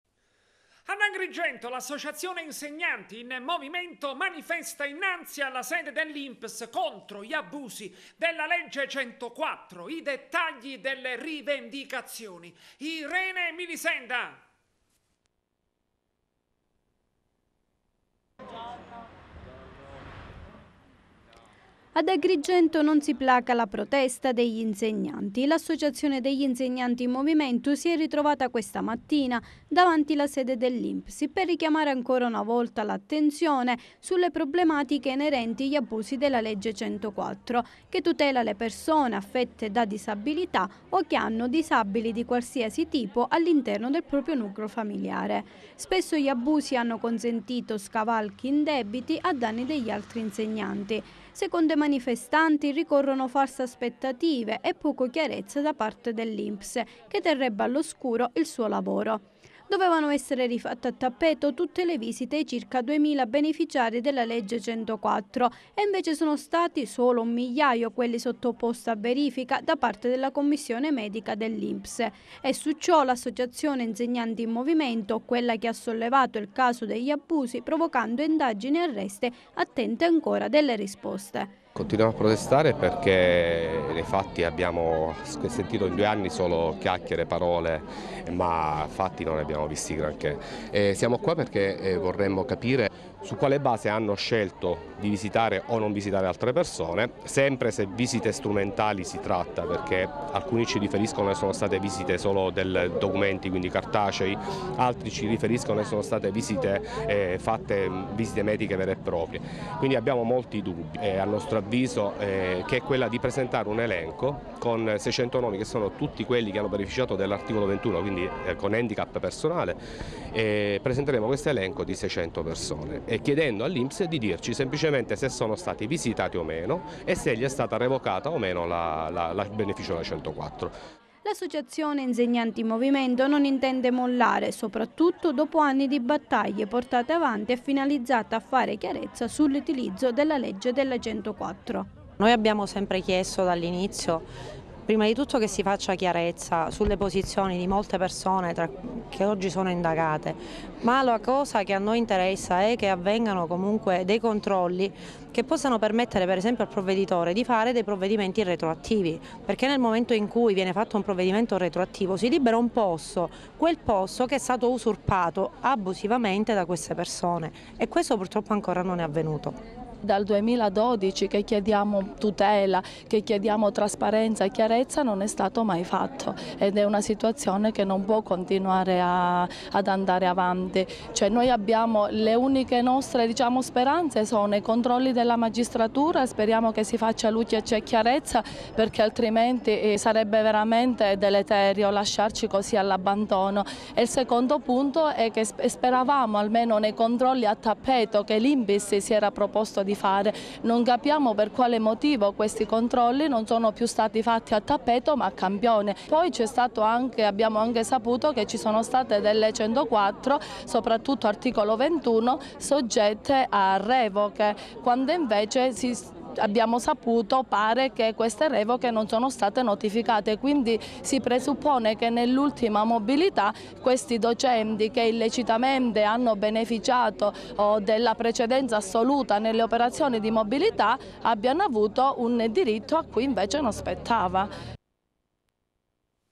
Teleacras - Agrigento, protesta 104 all' Inps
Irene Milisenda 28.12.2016
Ad Agrigento l’ associazione “Insegnanti in movimento” manifesta innanzi alla sede dell’ Inps contro gli abusi della legge 104. I dettagli delle rivendicazioni. Irene Milisenda.

                                   MILISENDA
Ad Agrigento non si placa la protesta degli insegnanti. L’associazione degli “Insegnanti in Movimento” si è ritrovata questa mattina davanti la sede dell’Inps per richiamare ancora una volta l’attenzione sulle problematiche inerenti gli abusi della legge 104, che tutela le persone affette da disabilità o che hanno disabili di qualsiasi tipo all’interno del proprio nucleo familiare. Spesso gli abusi hanno consentito scavalchi indebiti a danni degli altri insegnanti. Secondo i manifestanti ricorrono false aspettative e poca chiarezza da parte dell’Inps, che terrebbe all’oscuro il suo lavoro. Dovevano essere rifatte a tappeto tutte le visite ai circa 2mila beneficiari della legge 104. E invece sono stati solo un migliaio quelli sottoposti a verifica da parte della commissione medica dell’Inps. E su ciò, l’ associazione “Insegnanti in movimento”, quella che ha sollevato il caso degli abusi provocando indagini e arresti, attende risposte… Intervista a Lorenzo Navarra al Vg…
L’associazione “Insegnanti in movimento” non intende mollare soprattutto dopo anni di battaglie portate avanti e finalizzate a fare chiarezza sull’utilizzo della legge delle 104… Interviste al Vg…